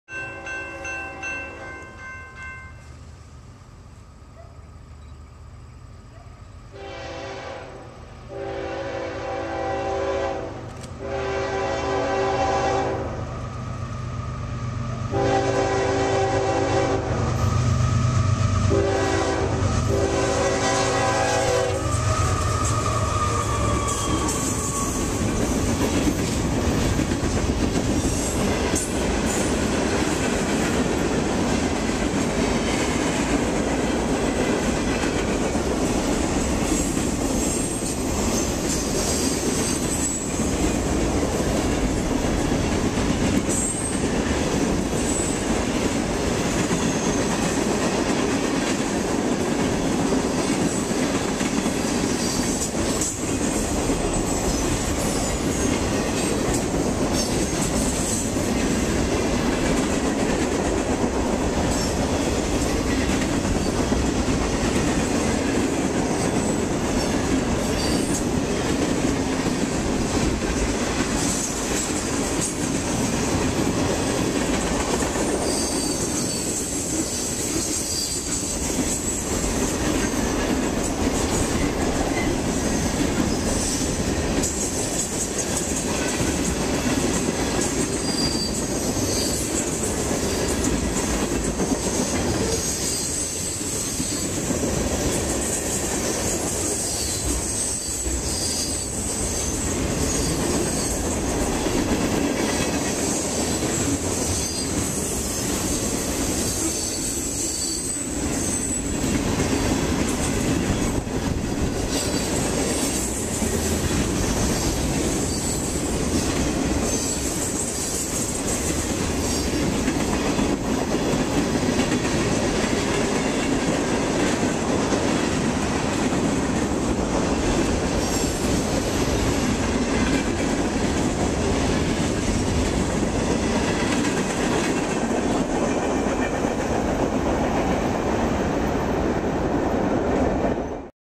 Westbound CSX - New Market
29 September 2012 - On my way back from the North East Fallen Flag Railroad Prototype Modelers meet, a short stop and I caught this FAST, CSX freight on CRSA. Lots of coil cars, tank cars and short covered hoppers.